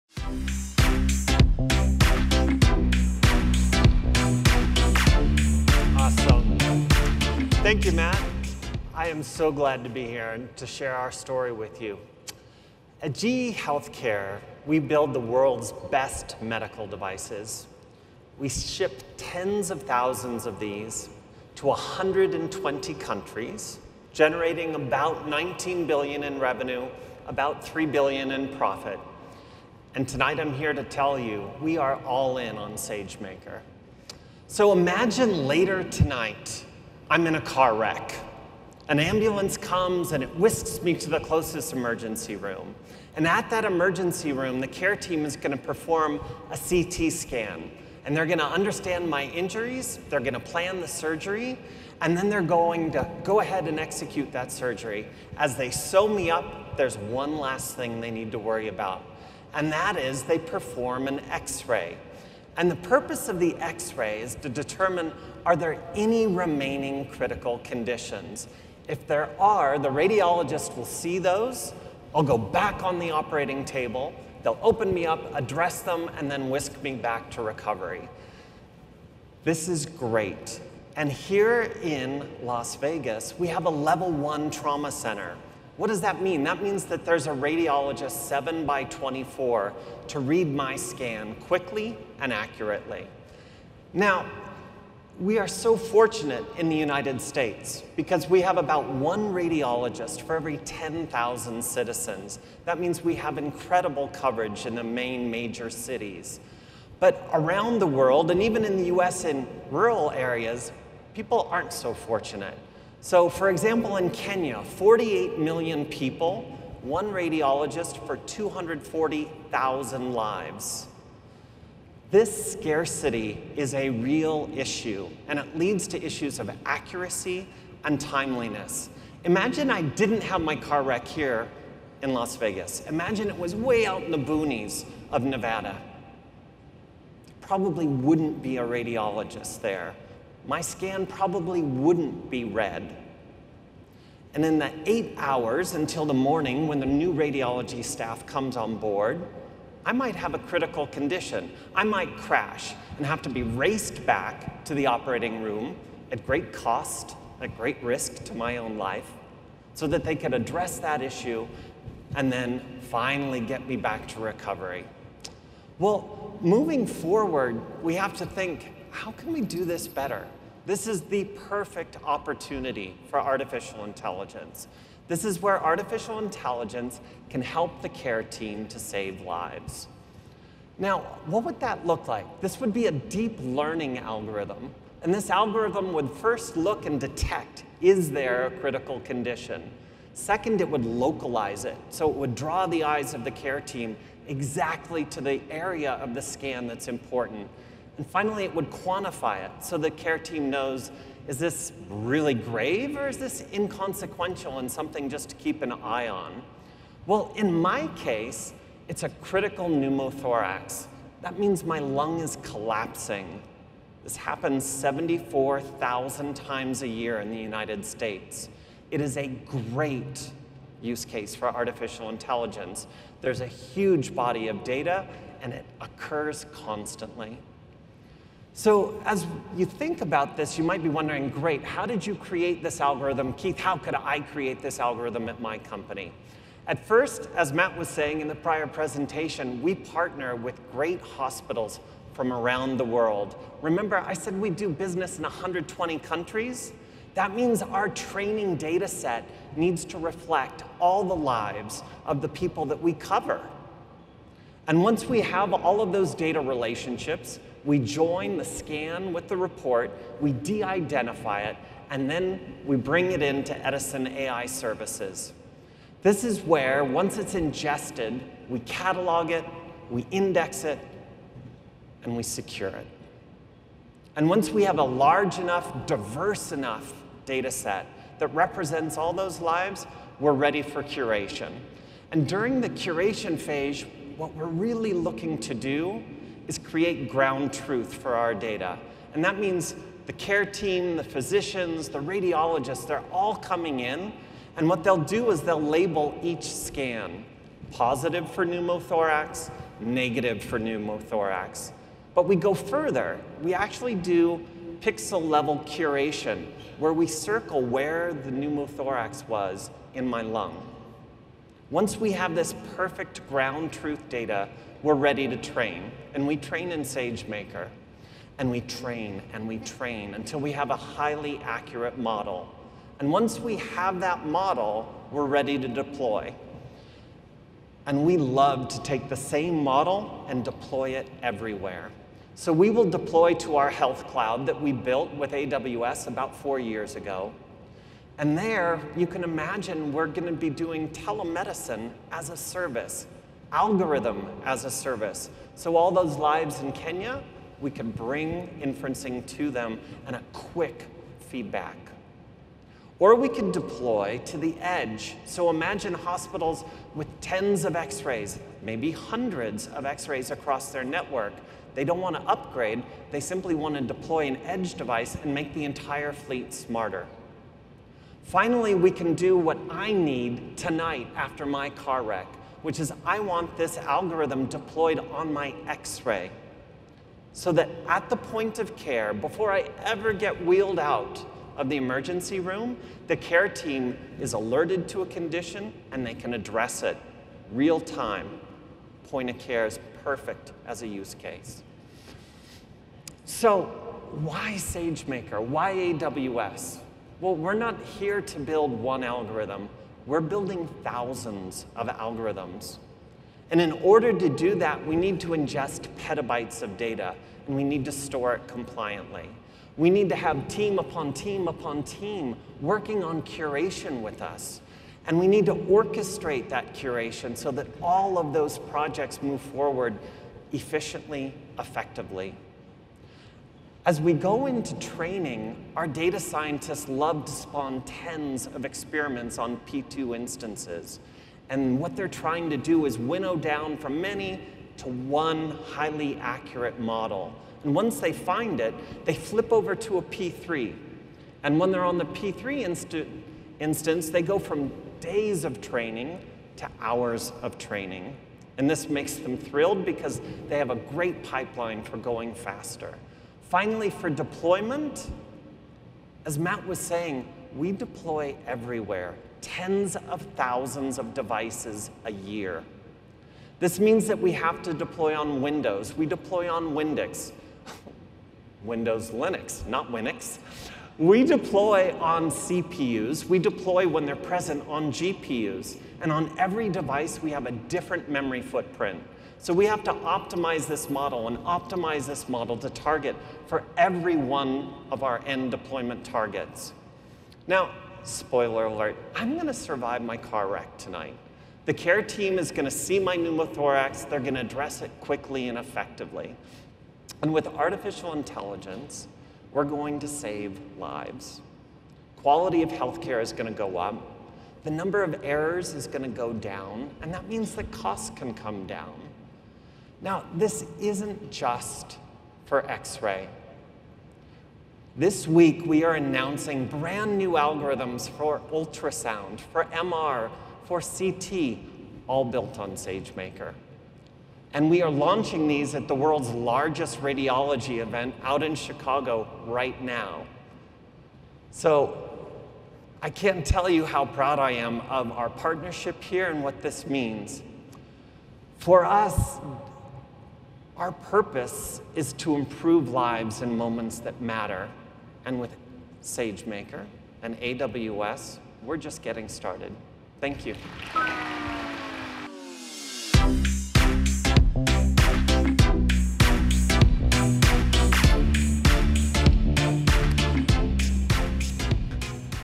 AWS re:Invent 2018: Keith Bigelow, GM and SVP at GE Healthcare, Speaks at Monday Night Live
GE Healthcare is using Amazon SageMaker to build, train, and deploy machine-learning models that help care providers identify health issues faced by people around the world. GE Healthcare manufactures and distributes diagnostic imaging equipment and software that improves patient outcomes by sharing medical-image data among medical practitioners. GE Healthcare uses AWS and Amazon SageMaker to ingest de-identified data, store that data compliantly, orchestrate curation work across teams, and build deep learning algorithms. Using Amazon EC2 P3 Instances, GE Healthcare reduced the time to train its machine-learning models from days to hours, allowing the company to deploy models more quickly. Keith Bigelow, SVP of Edison Platform Strategy at GE Healthcare, spoke onstage at re:Invent 2018.